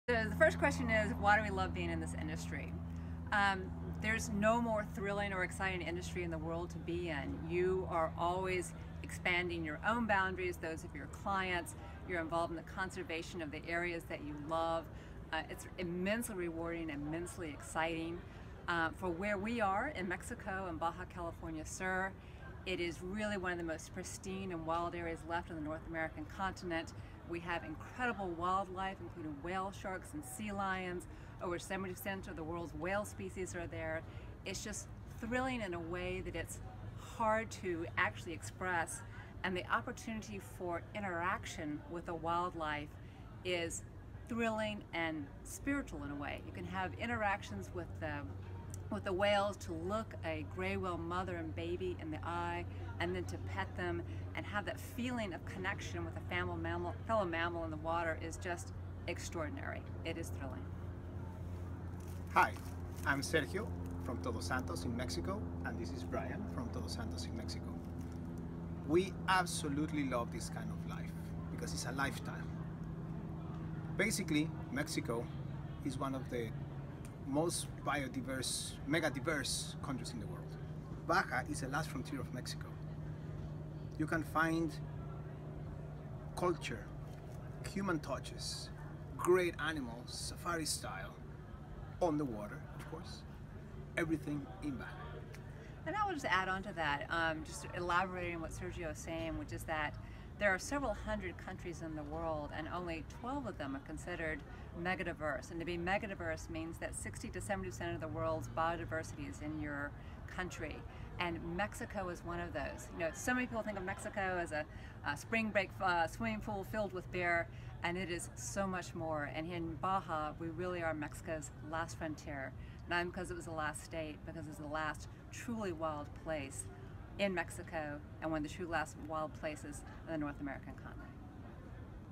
Bryan and Sergio - Mexico
The dynamic duo who are behind the MTS Baja adventures! Bryan and Sergio share their passion and expertise for this beautiful frontier, located just a short flight away from the lower 48.